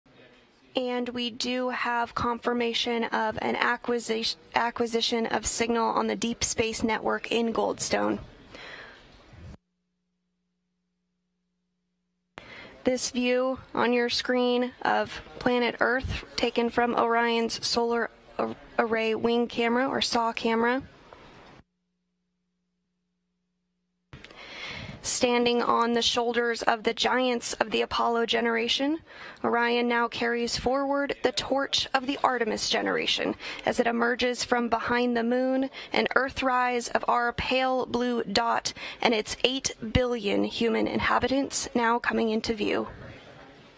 See Earthrise for the First Time from Orion Spacecraft
Watch Orion capture Earth rising as a tiny blue dot for the first time.